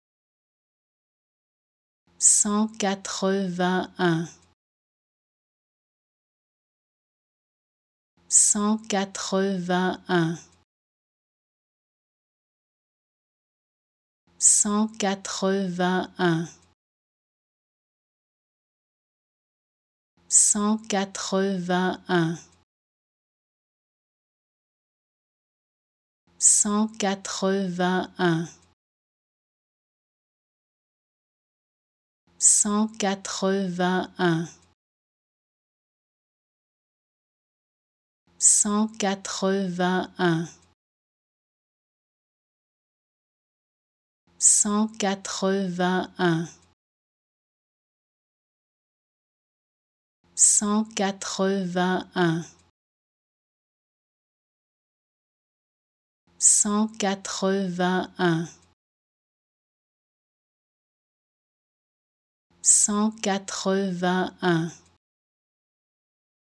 How to Pronounce ''Cent quatre-vingt-un'' (One hundred and eighty-one) Correctly in French
Learn how to say and properly pronounce ''Cent quatre-vingt-un'' in French with this free pronunciation tutorial. Apprenez à prononcer des mots en français grâce à nos tutoriels.